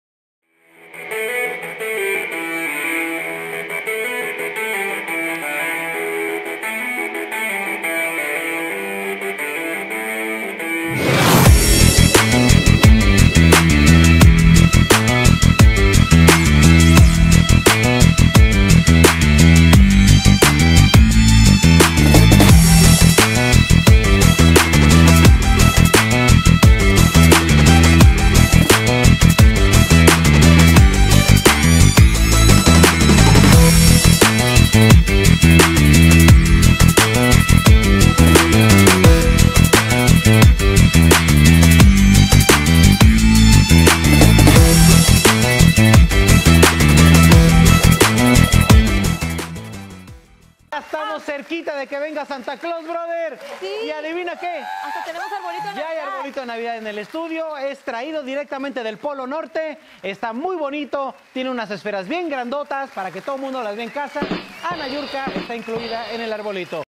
¿Por qué Monterrey sí ganará esta final? | Copa MX | Telemundo Deportes
Video oficial de Telemundo Deportes. Este jueves, Monterrey y Pachuca se medirán en la final de la Copa MX y aquí te contamos por qué los Rayados se coronarán.